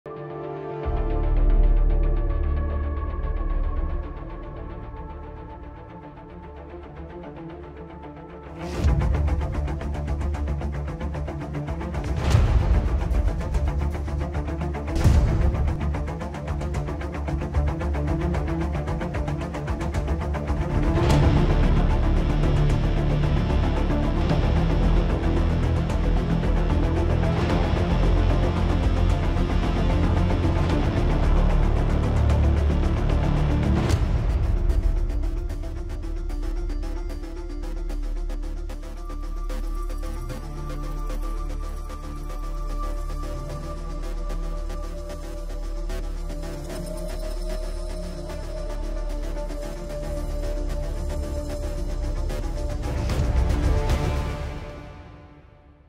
The GLYPH CRADLE Theory is Awesome and would Explain Everything (Eclypse Event)
The Fortnite Glyph's might have been cracked. The teasers and speculation for the Eclipse event fit very well with the Theory that the glyphs show Cubetown as well as the Cube Cradle.